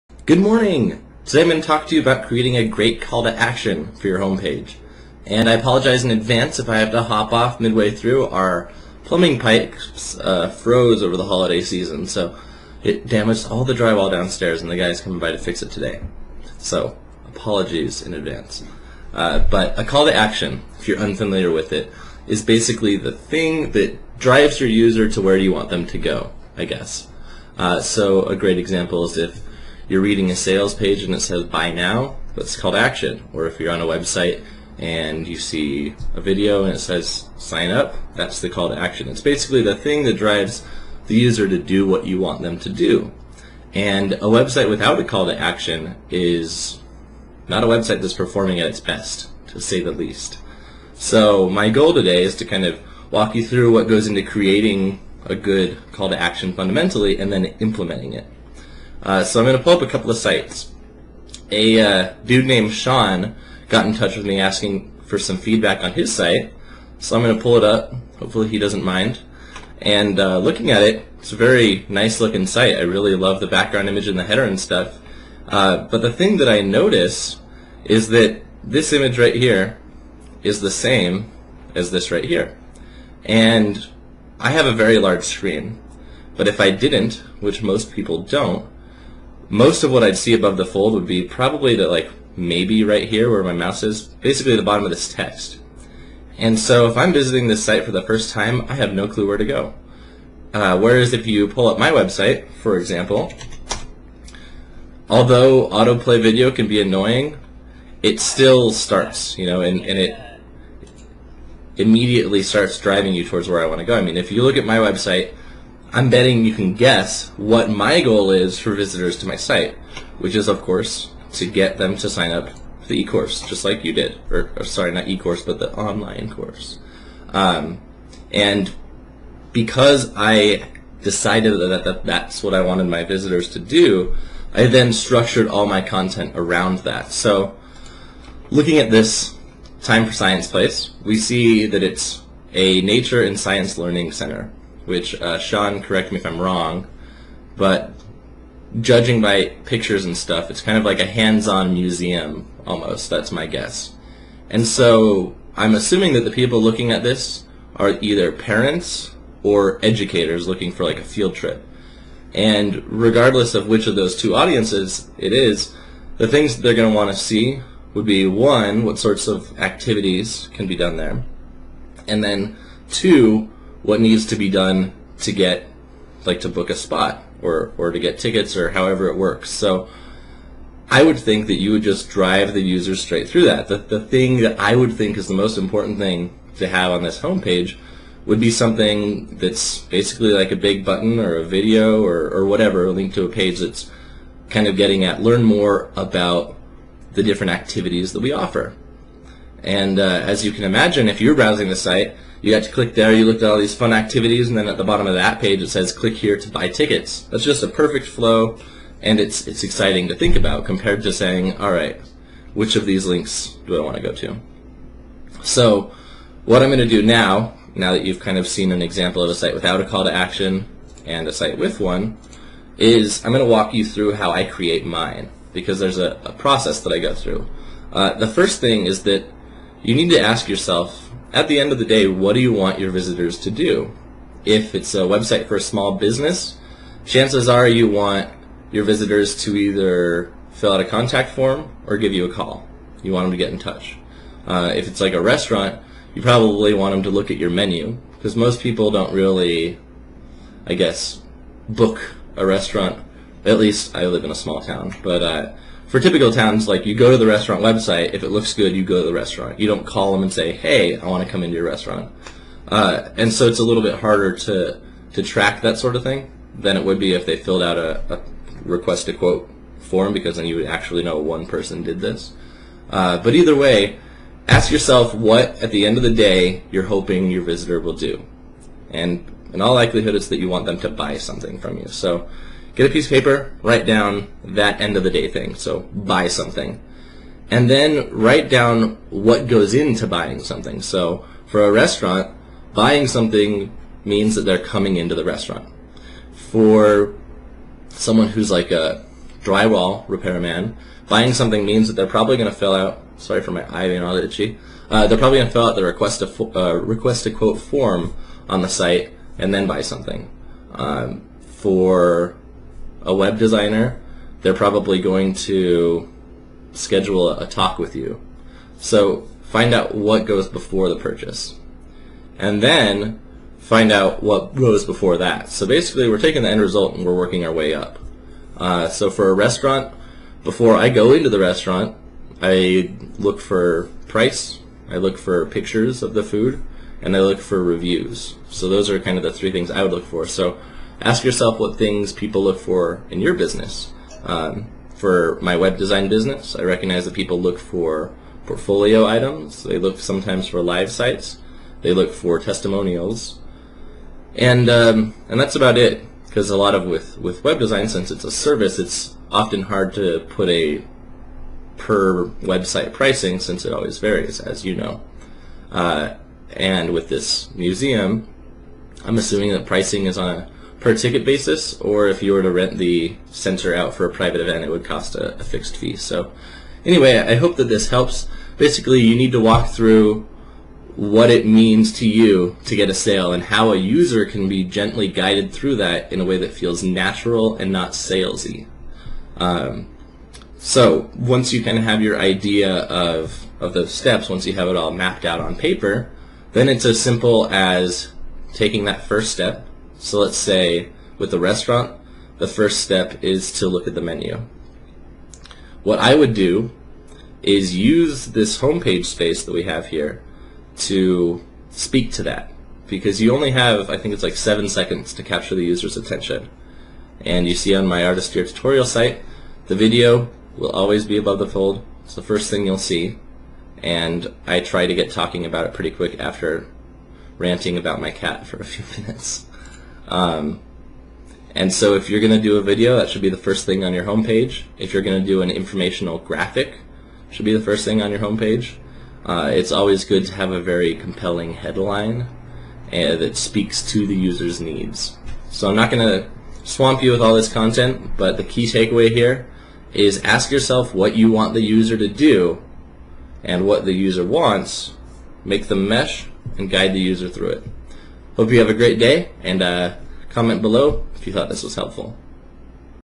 How to create a strong call-to-action for your website
The call-to-action (CTA) is the thing that drives your website traffic to where you want them to go. Without a strong CTA, your website will not be living up to its full potential.

In this video, I show you the process that I go through to come up with a great CTA, and them implement it on my site.